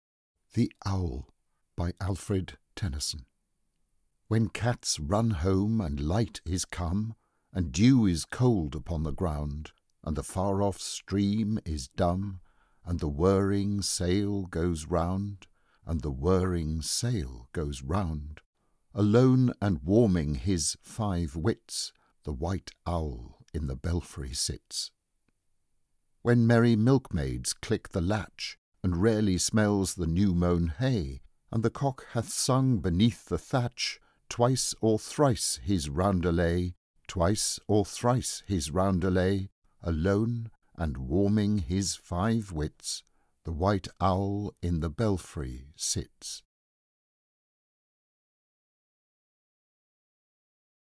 The Owl - a poem by Alfred, Lord Tennyson
Read for The Healing Voice by Colin WD McLean